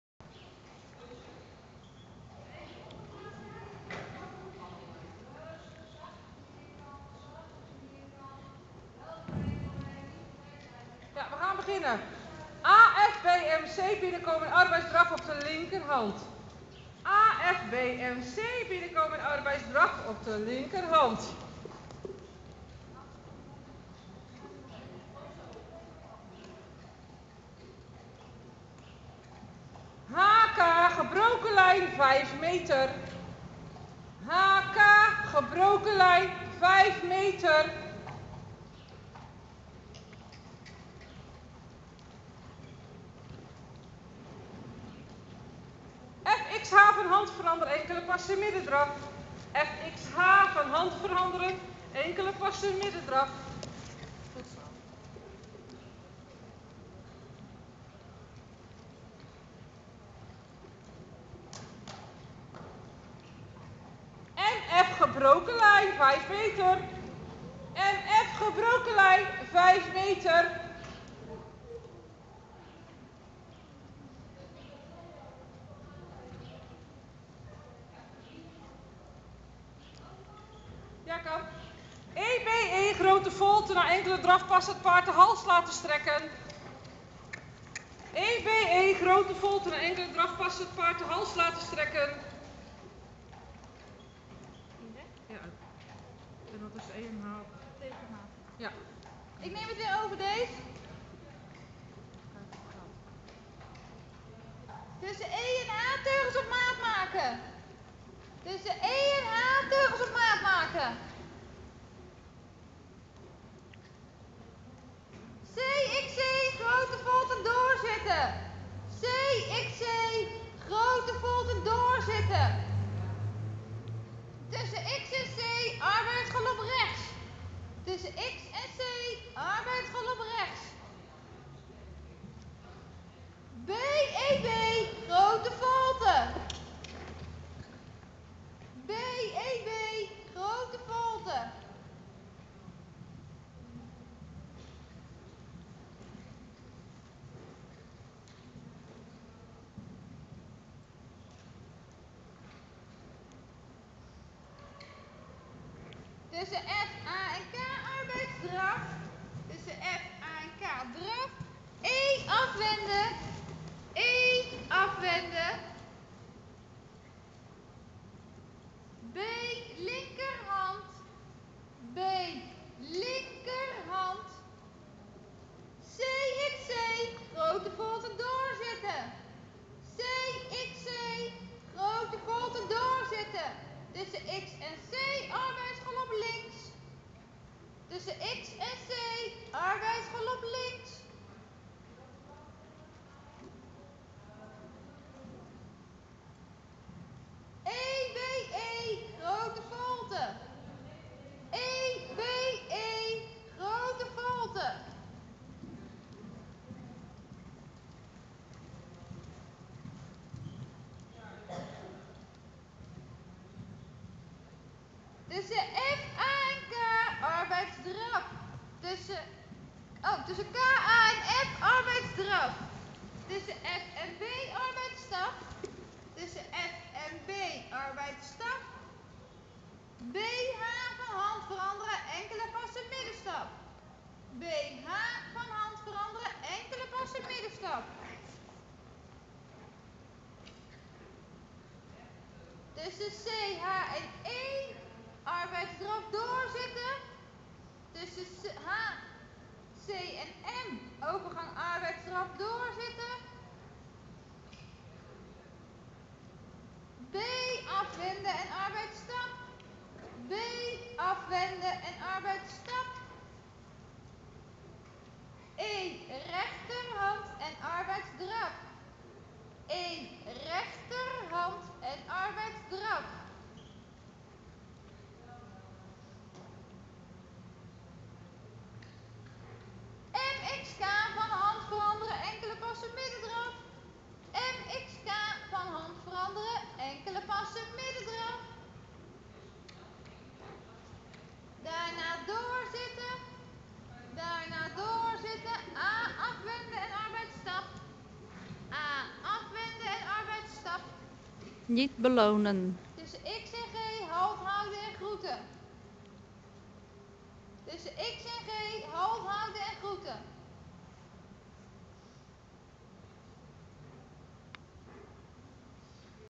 Paarden Centrum sliedrecht - daisy & zennit onderlinge wedstrijd 2011
2x 1ste met 196 en 197 punten!
op pc sliedrecht